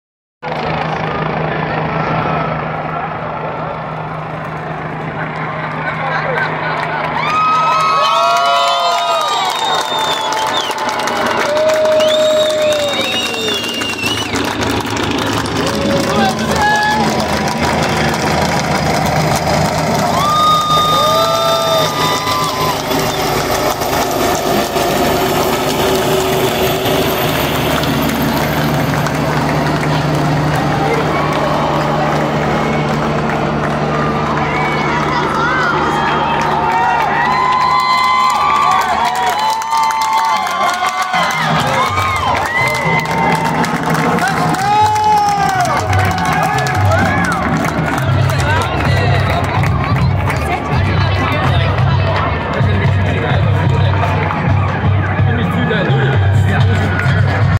Chopper flyover at Barron Collier - Naples football game.
Helicopter flyover at our local high school football game just prior to speech and coin flip by Ron DeSantis.